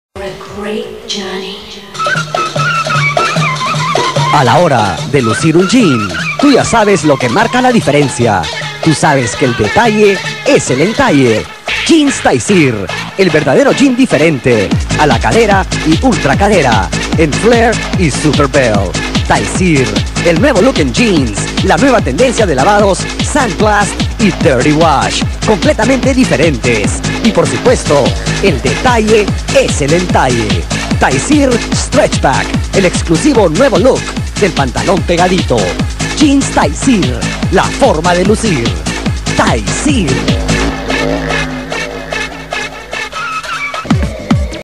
Comercial 1.wmv
Comercial en radio Doble Nueve alla por los años 90s, disculpen por el sonido pero la cinta ya estaba maltratada, pero se puede oir. saludos